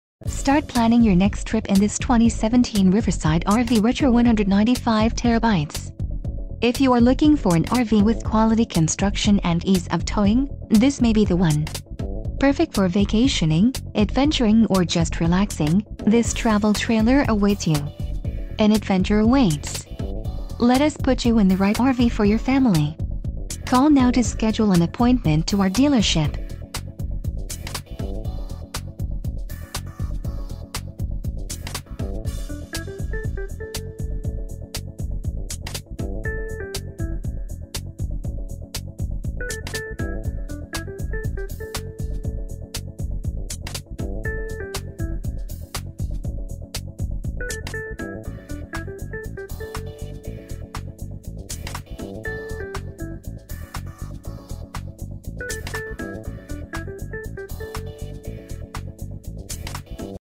2017 Riverside RV Retro 195TB - Chilhowee RV Center - Lou...
This 2017 Retro travel trailer by Riverside RV is the model 195TB. This unit has a twin bed.SPECIAL LIMITED OFFER